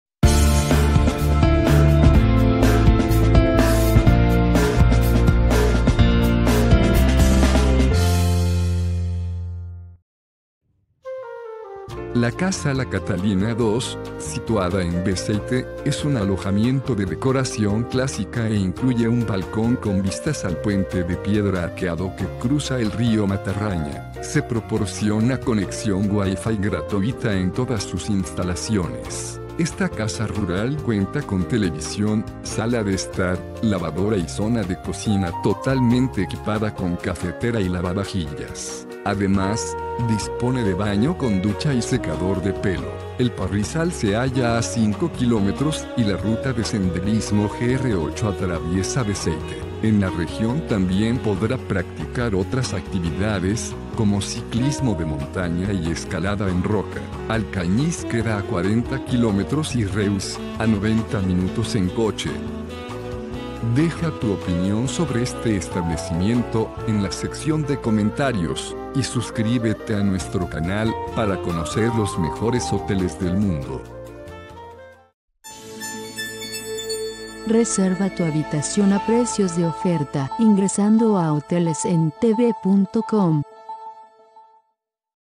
Casa La Catalina II, Apartamento en Beceite
Casa La Catalina II dispone de 3 unidades provistas de ducha, tv y secador de pelo. La propiedad se encuentra localizada en una atractiva área de Beceite.